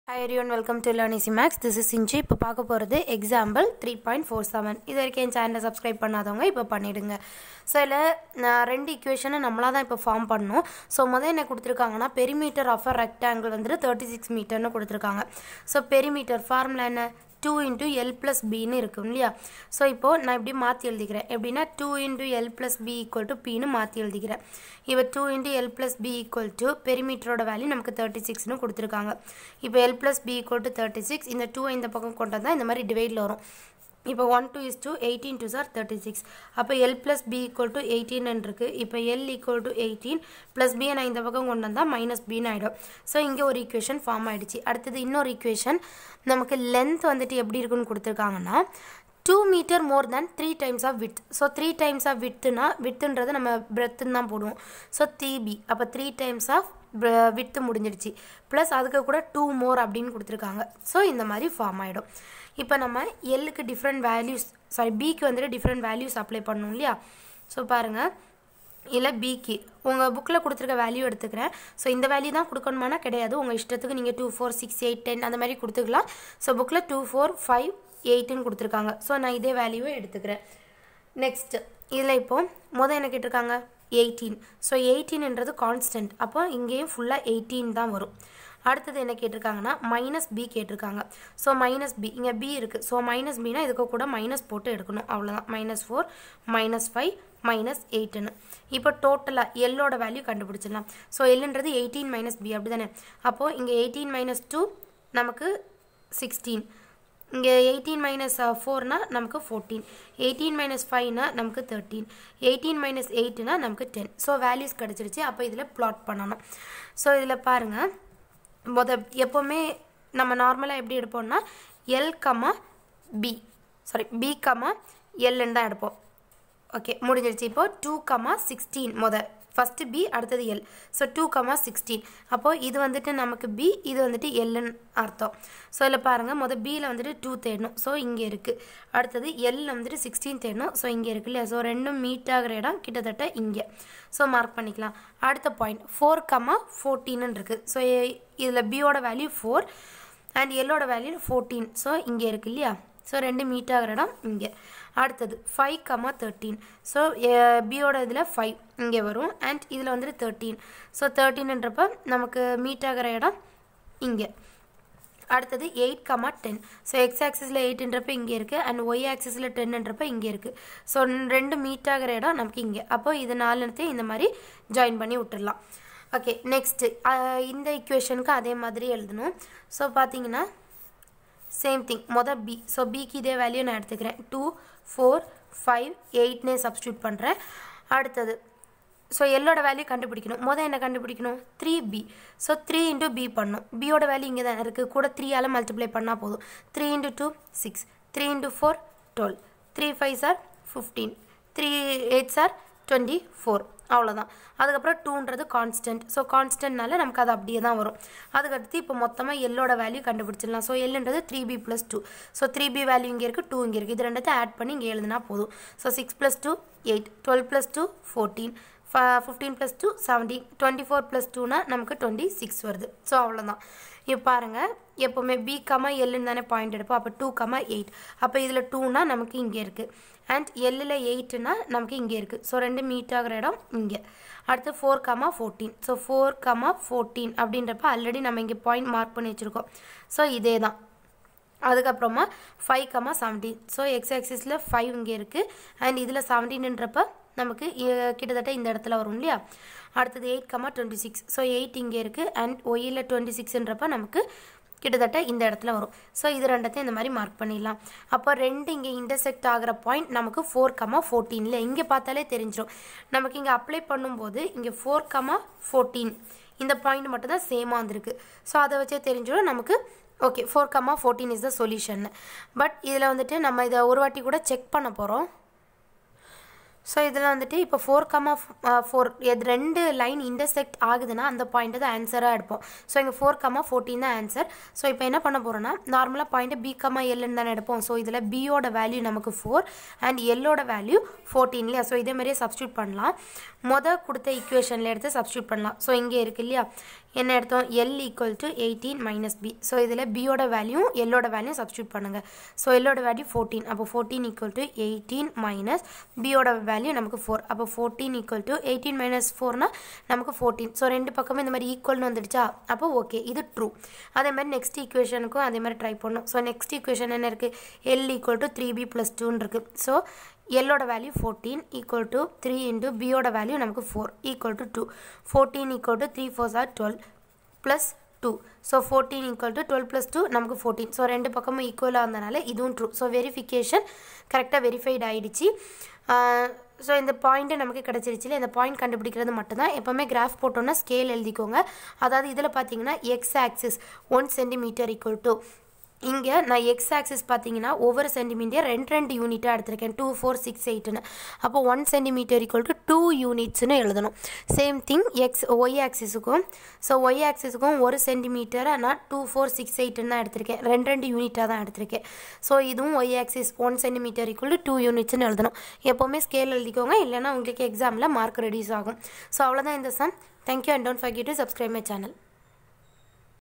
9th Maths Chapter 3 Example 3.47 Samacheer State board Tamil English medium
Hello Everyone this is Sinju. Here I have explained Example 3.47 from Chapter 3 for 9th std. Also I have Explained the problems in a more simple and easy method. Watch the video and then try to solve the problem on your own. It will surely help you. All the Best to score full marks in maths